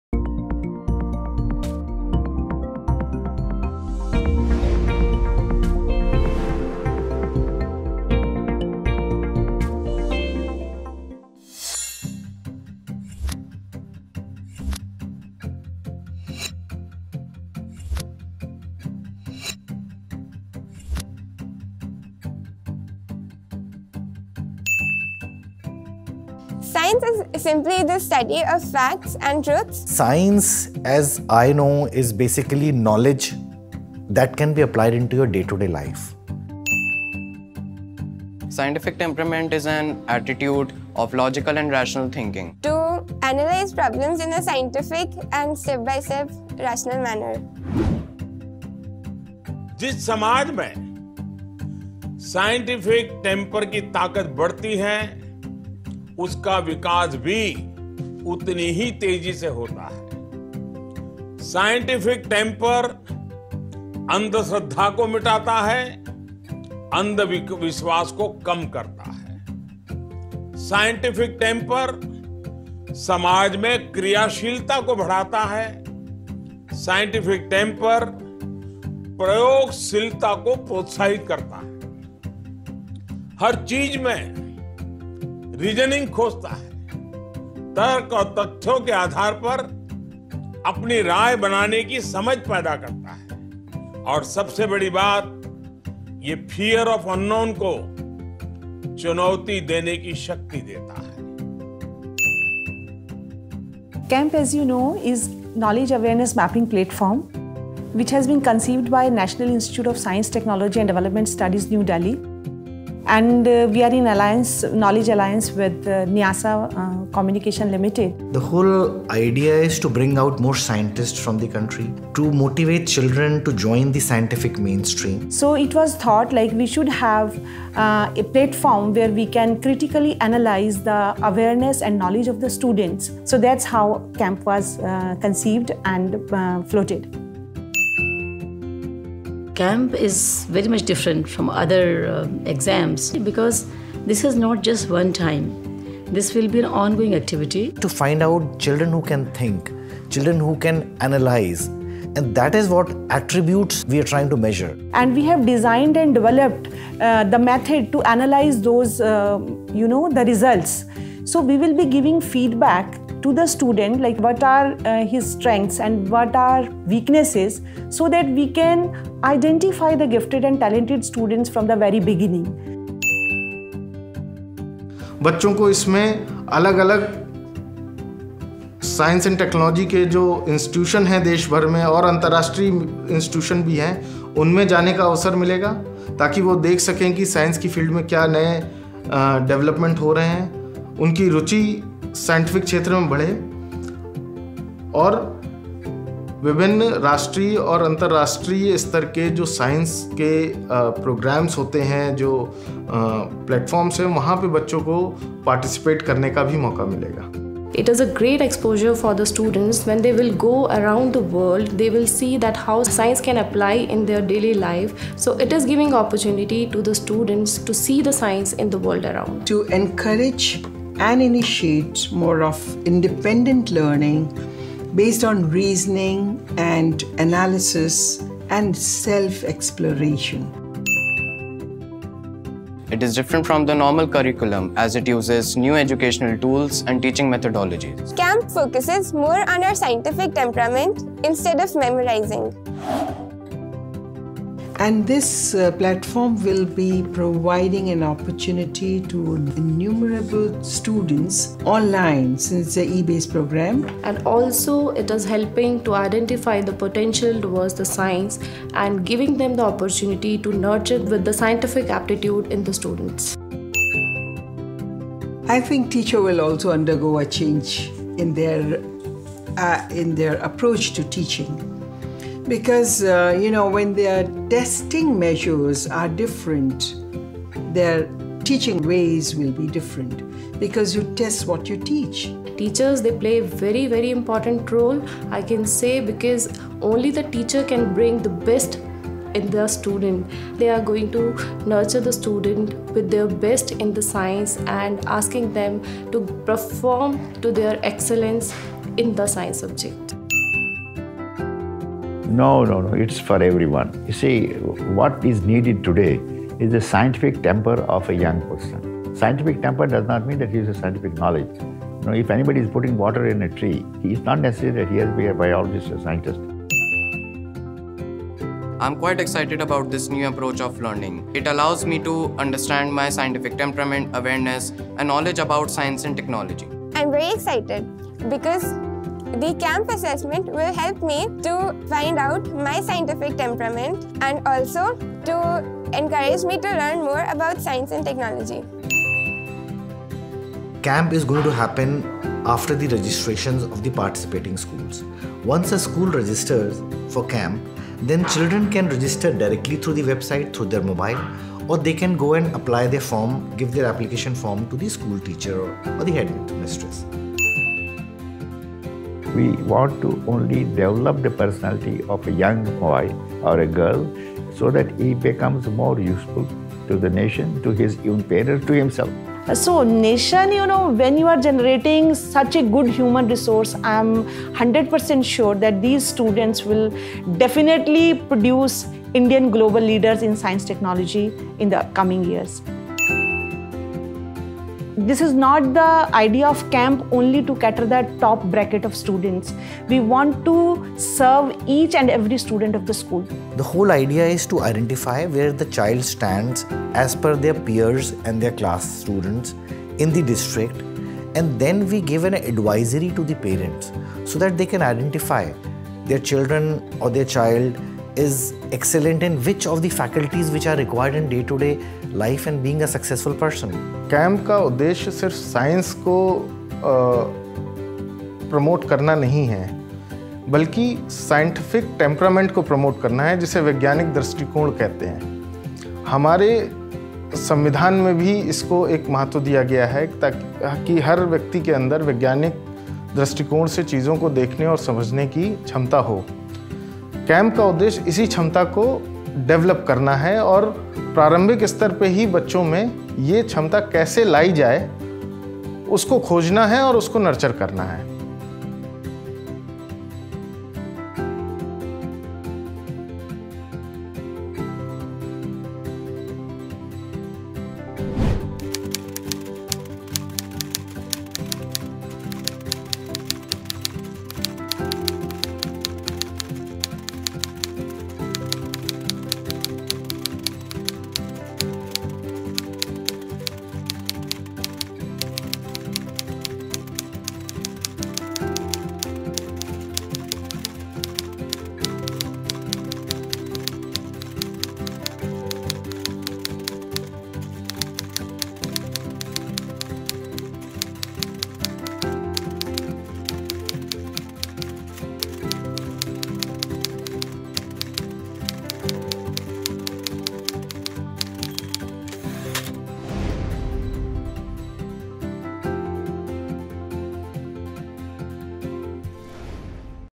KAMP | TESTIMONIAL VIDEO | PARIVARTAN PICTURES
Who could explain KAMP better than the students, teachers and the people behind it's inception. An initiative by CSIR-NISTADS to infuse scientific temperament in the generation now and the ones to come.

Team Involved:

Client - Nysa Communication Private Limited
Client's Team - Puneet Kumar, Ashish Mittal, Arika Mathur
Project - Knowledge and Awareness Mapping Platform

Production House: Parivartan Pictures
Questionnaire formed by - Rakesh Ranjan
Executive Producer - Rishabh Kohli
Project Supervision - Abid khan
Director - Manvi Duggal
Creative Director - Prashant Upadhyay
Cinematographer - Jitender Singh

Editor - Kedar Mainkar
Logo Design - Nisha and Ved Mahajan
Sound Designer - Kay Kundan
Music Composer - Chirag Soni